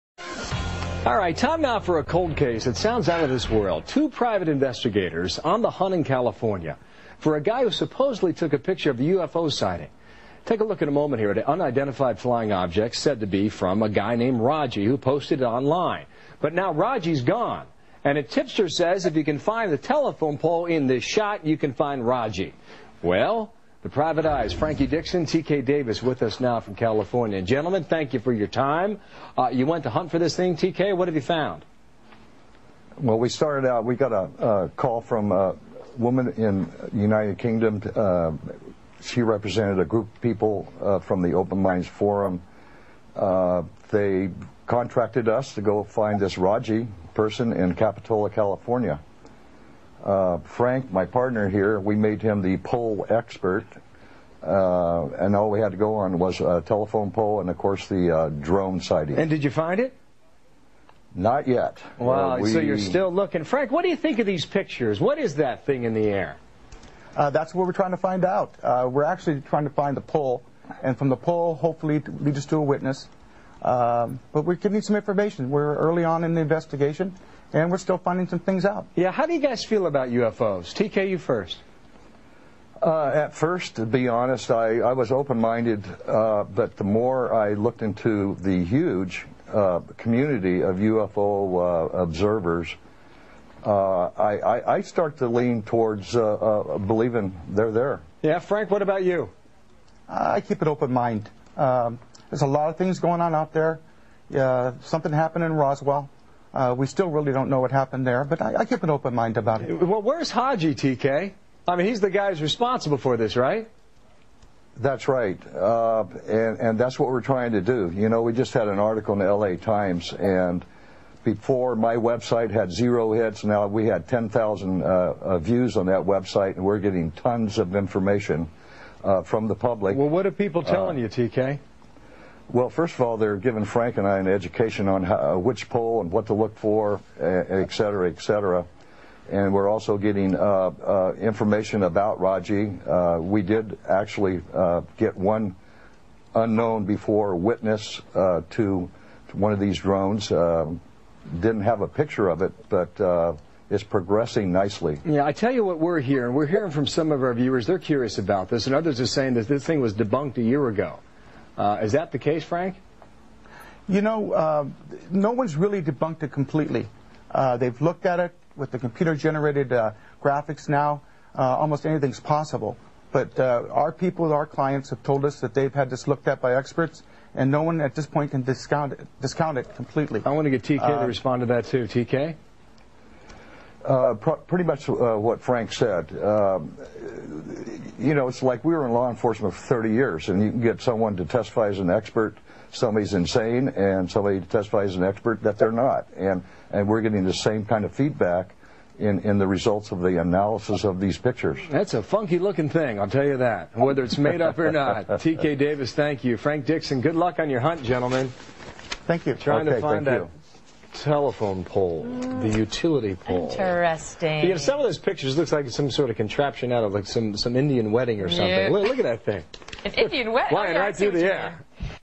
Capitola Dragonfly Drones Spotted On Fox News [HQ]
Short Fox News Report on the two Private Investigators charged with locating the utility pole and the hopefully Raj, the photographer of the Dragonfly Drone. Commissioned by OMF, perhaps the end is nearing for a collection of some of the most intriguing UFO reports of recent times.